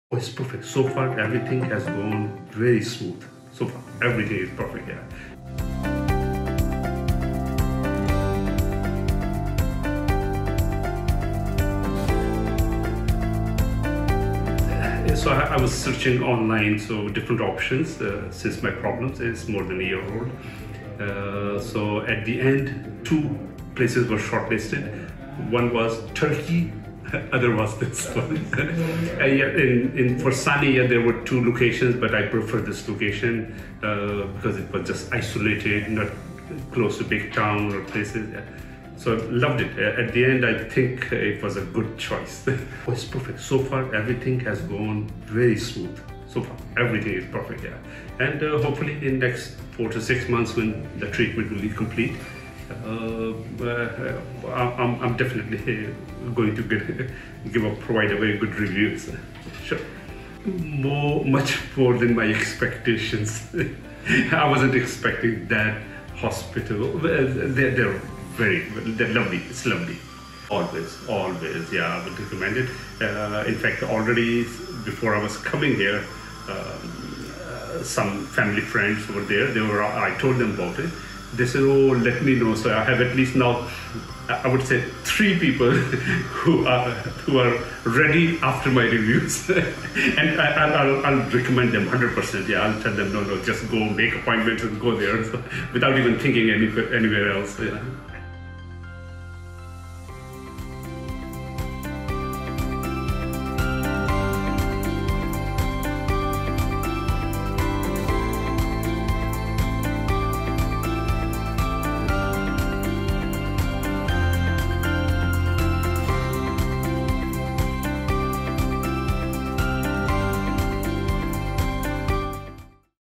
All on 4 Mexico Dental Experience - Sani Dental Group
"Everything has gone very smoothly, It's been perfect!"

After an online search for the Best Dental Clinic to go abroad, our dear patient Zaid decided to visit Sani Dental Group for his dental needs. Out of our three state-of-the-art dental facilities, he ends up choosing our Playa del Carmen clinic to relax during his stay next to the Riviera Maya.

During his visit, he was able to start his All-on 4 treatment, as well as 3 Zirconia Crowns, an Extraction, and a Bone Graft.

We can't wait to see Mr. Khalid again in the next 6 months to continue with his All on 4 Treatment, but until then, we welcome you to make the life-changing decision to change your smile!

📍 Los Algodones, Mexico.
📍 Playa del Carmen, Mexico.
📍 Cancun, Mexico coming soon!

0:00 Overall Experience
0:15 Choosing Sani Dental Group
0:42 All on 4 Dental Treatments
1:10 Would you recommend Sani Dental Group?